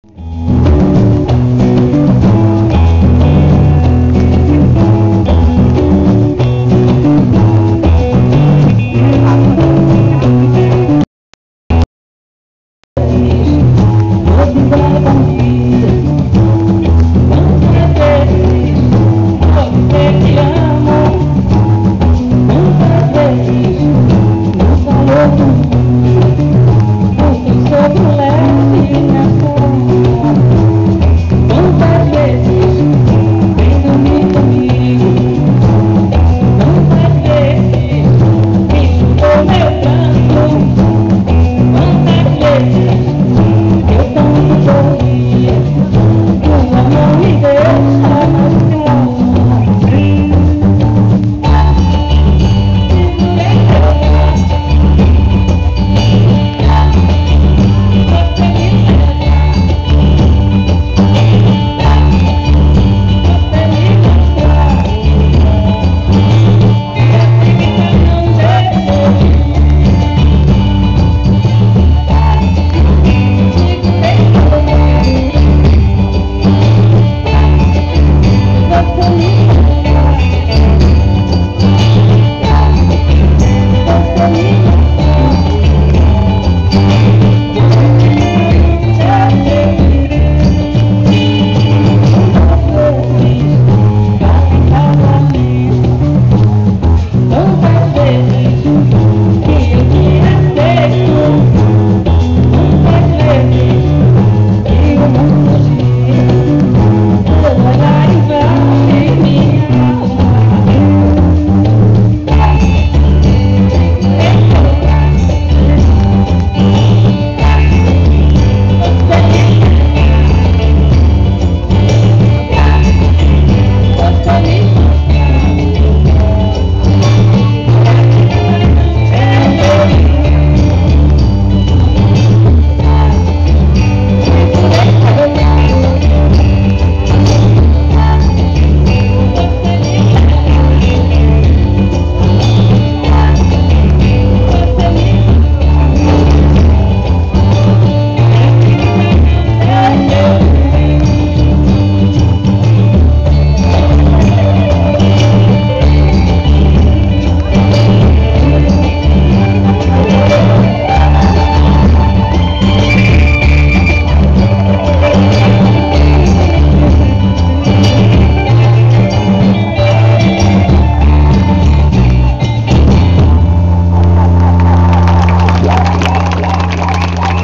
02.01.2010 [parte 2/3] Alessandra Leão na Praça da República
Para o Ciclo Natalino do Recife

Santo Antônio
Recife - PE
Brasil